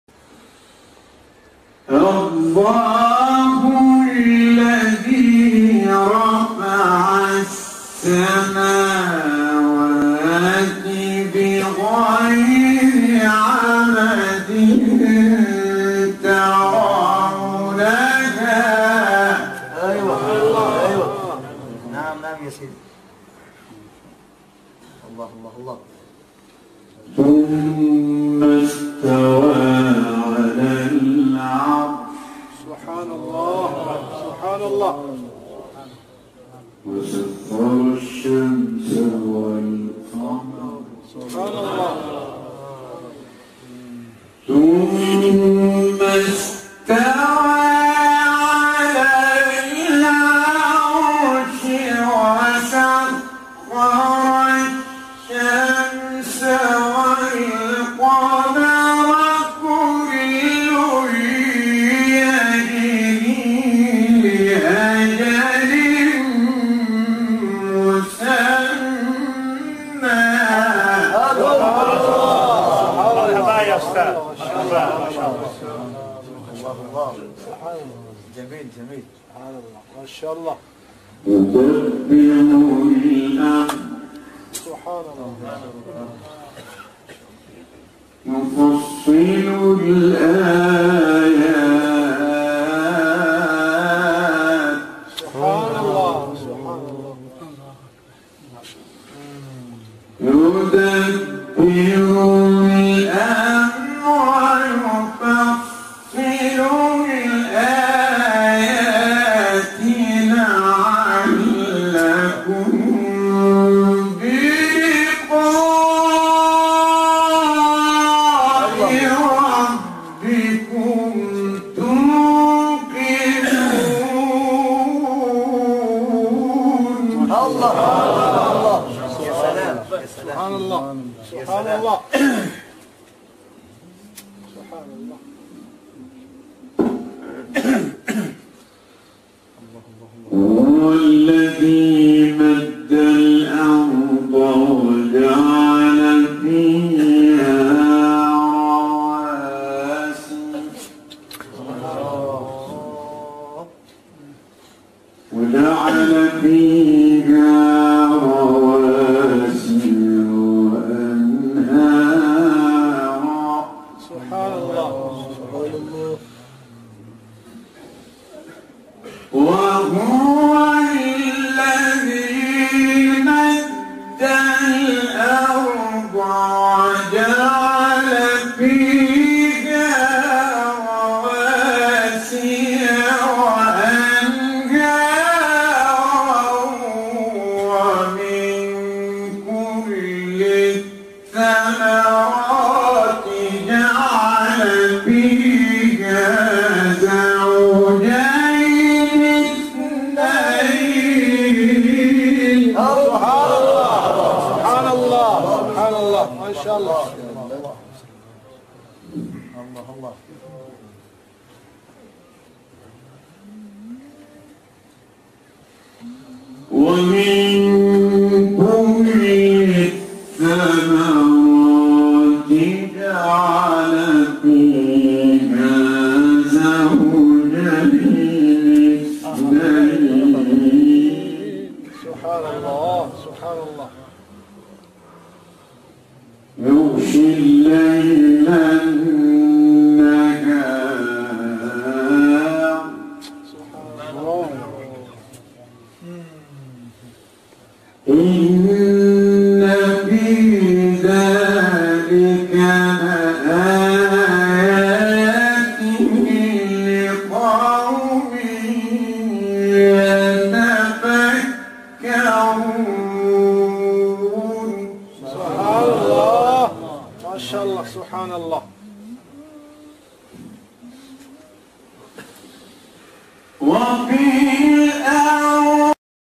الشيخ سيد متولي | اللَّهُ الَّذِي رَفَعَ السَّمَاوَاتِ بِغَيْرِ عَمَدٍ تَرَوْنَهَا | بريطانيا 🇬🇧
Sheikh Mutawalli (Rah) in Nelson, UK - August 2010.